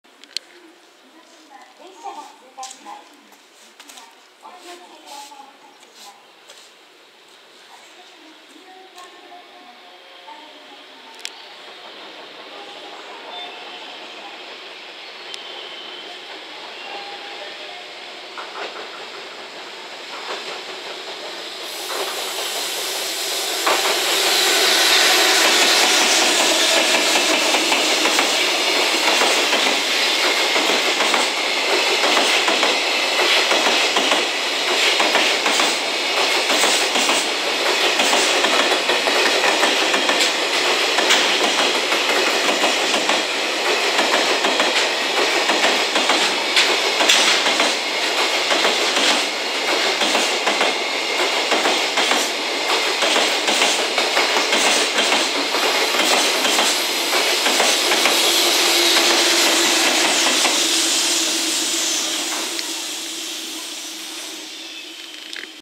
ムド+後補機付 山陽本線 上り 貨物列車 2072レ EF210-315号機 牽引 EF210-168号機+コンテナ21B + EF210-301 JR貨物 海田市 2019.12.17 01554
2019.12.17
全検明け EF210-168号機の回送